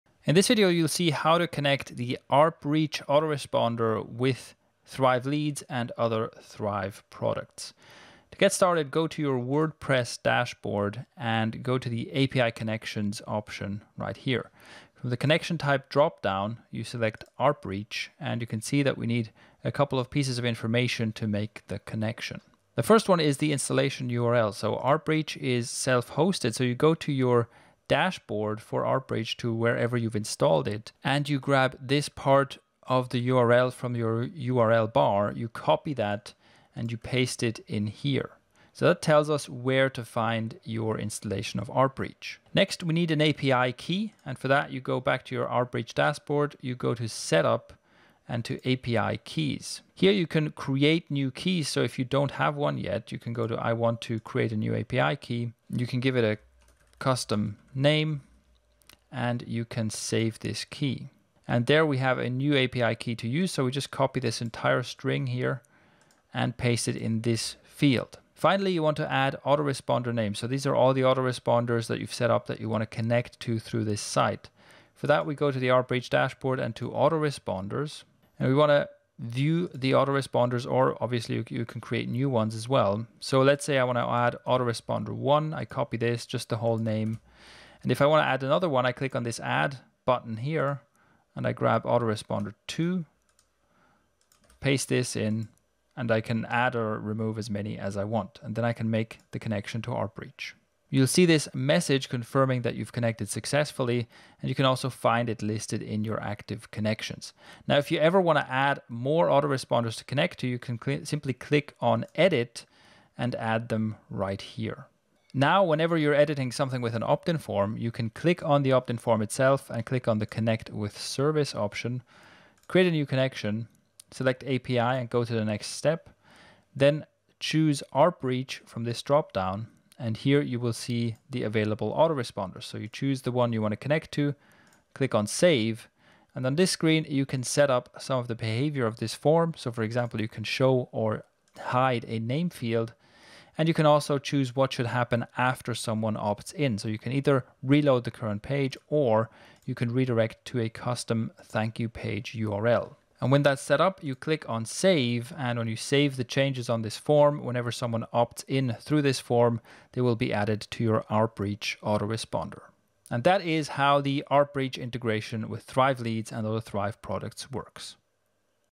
How to Use arpReach with the Thrive Leads WordPress Plugin
A quick tutorial showing how you can use the arpReach email marketing system together with the Thrive Leads list-building plugin for WordPress.